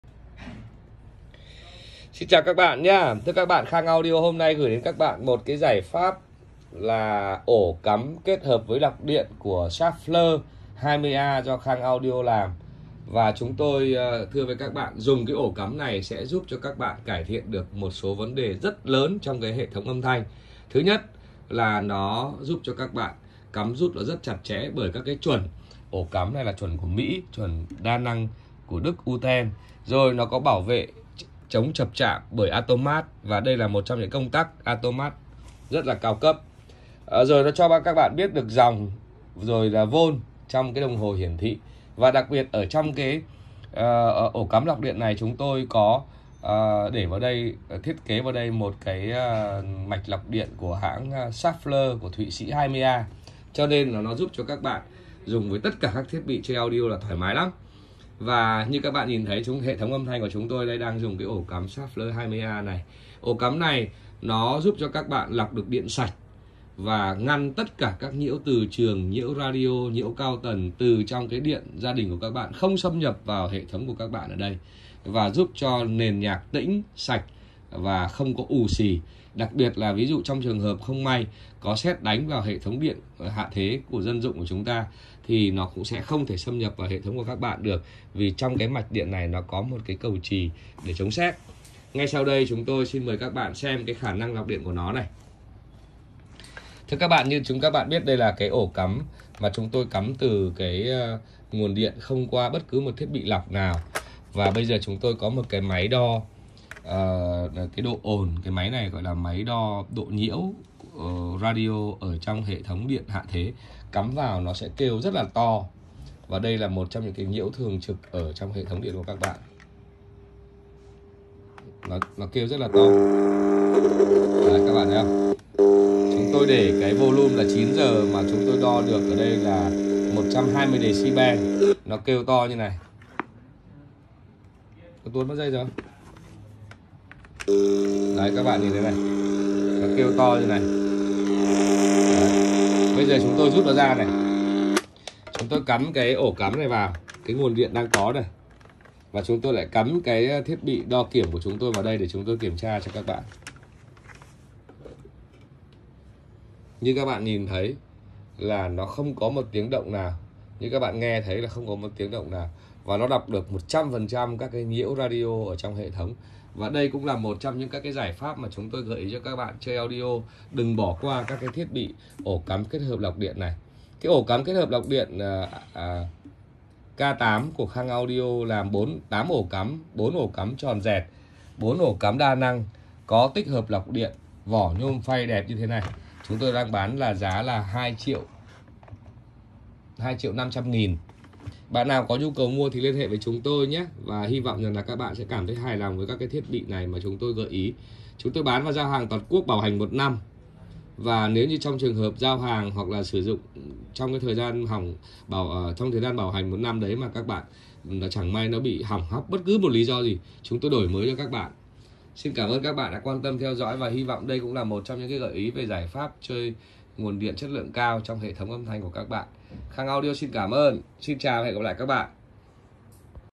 Giải Pháp Nguồn Điện Audio Lọc Nhiễu Âm Thanh Sạch Nhạc Tĩnh Ổ Cắm Lọc Điện K8 2.5 Triệu
Nguồn điện có vai trò rất quan trọng đối với chất lượng âm thanh và độ bền của các thiết bị chơi audio như đầu đọc, amply. Các vấn đề như nguồn điện đủ nhưng phích cắm lỏng, chập chờn, nguồn điện nhiễu hay không có tiếp địện... là những yếu tố bất lợi cho chơi audio. Người chơi audio muốn mua những thiết bị ổ cắm lọc nguồn điện của một số hãng Hi-end trên thị trường thì quá đắt đỏ, mua những thiết bị lọc điện của Trung Quốc hàng giá rẻ thì không đảm bảo chất lượng, chóng hỏng.

Từ thực tiễn chơi audio và mong muốn quý khách mua được lọc điện chất lượng cao giá cả hợp lý, Khang Audio đã làm ra chiếc Lọc Điện K8 Schaffner Thụy Sĩ 20A đáp ứng được các yêu cầu của quý khách.
1. Vật liệu sử dụng làm vỏ ổ cắm lọc điện là nhôm phay anot tinh tế, hiện đại
2. Dây điện đi trong ổ cắm Lọc Điện K8 Schaffner Thụy Sĩ 20A là dây của Nhật chống cháy với đường kính dây 2,5mm
3. Lọc điện Thụy Sĩ Schaffner 20A qua sử dụng hàng tháo máy y tế
4. Phích cắm kết hợp 2 loại: 4 ổ cắm loại 3 chân dẹt và 4 ổ cắm loại đa năng của Đức giúp cho người sử dụng được đa dạng các loại phích cắm.
5. Đồng hồ hiển thị volt và ampe loại cao cấp.
6. Lọc Điện K8 được cơ cấu 1 Atomat làm công tắc bật tắt có chức năng chống chạm chập và bảo vệ thiết bị chơi audio của các bạn.
7. Chức năng: Lọc nhiễu từ trường, nhiễu radio, nhiễu cao tần, những loại nhiễu điện này ảnh hưởng bất lợi cho đối với chất lượng âm thanh của hệ thống audio